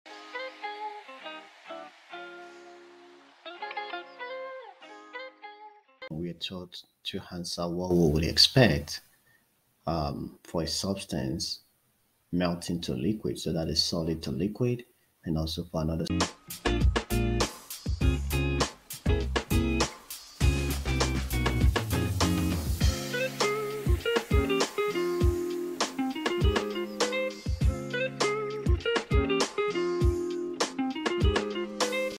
What would you expect to a substance melting to a liquid phase or subliming to a gas at 1 atm and r…
What would you expect to a substance melting to a liquid phase or subliming to a gas at 1 atm and room temperature? Can you give me brief answer only? Thank you, your help will be so much appreciated.

DiscoverthePowerofLiquids:BoostYourHealthandWellnessToday!